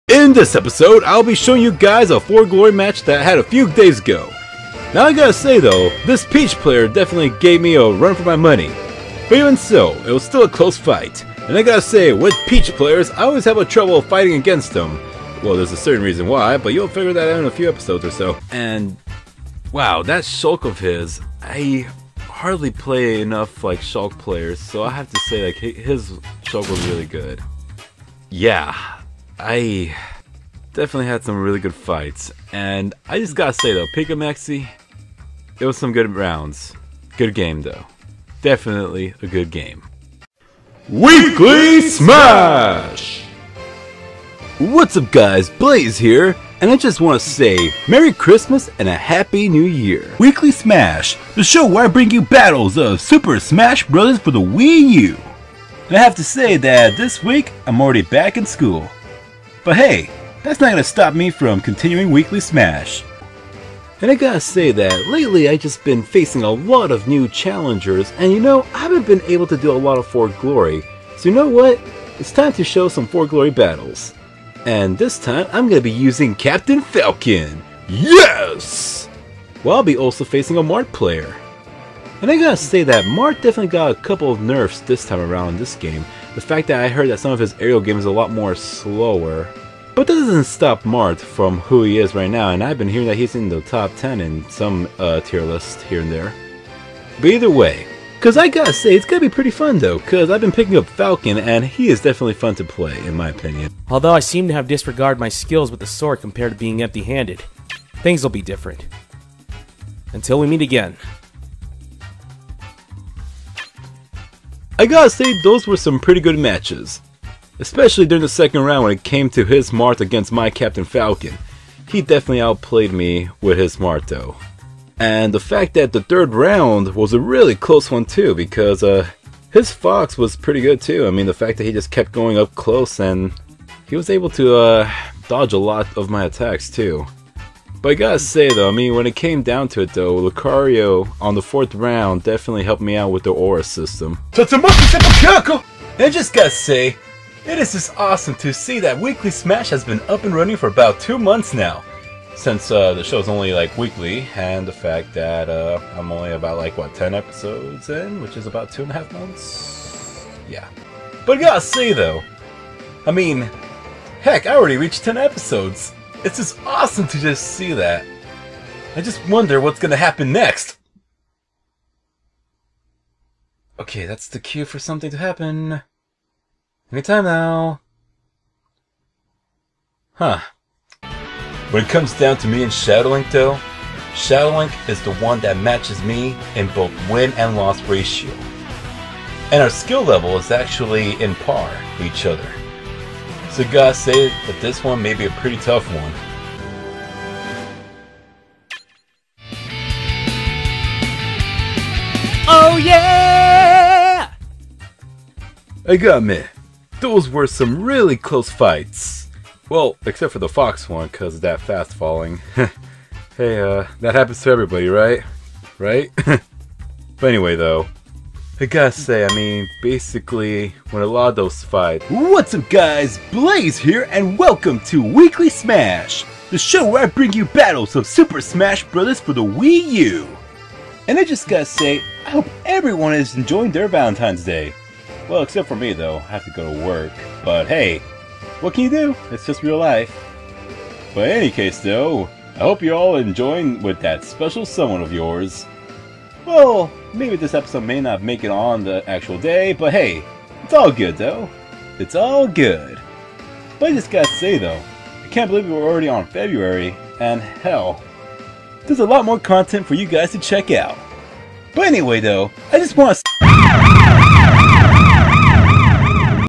Blaze's "I Just Gotta Say" Count
So here's the story:
Blaze1216j has an issue with saying the phrase "I just gotta say" alot. Like. ALOT ALOT.. so as a joke I've decided to show him how many times he's said that phrase in his miniseries "Weekly Smash" Let's find out together shall we? :D